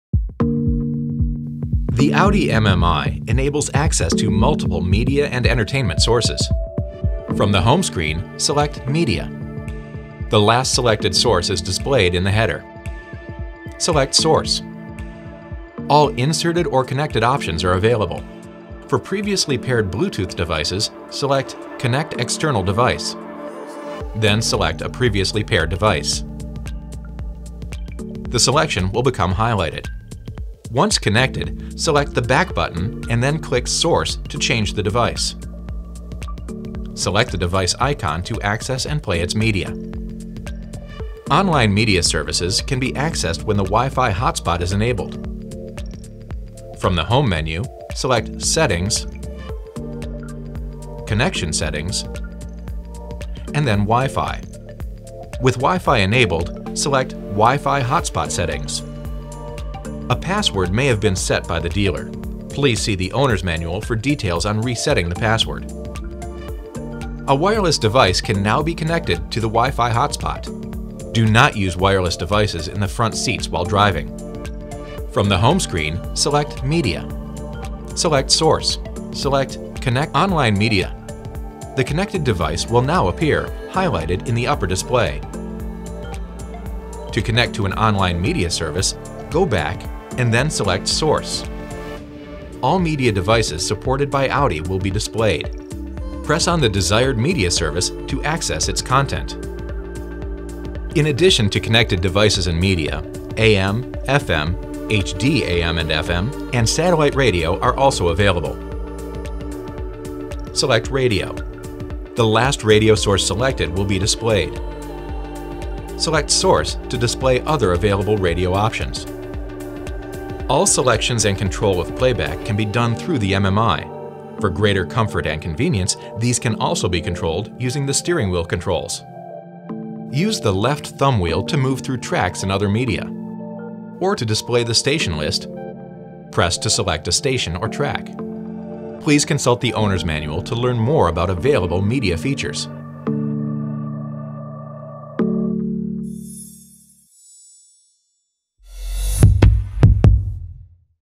Audi Tech Tutorial: Media and Entertainment
Discover the multiple media options available to you and how to access and operate them in the MMI.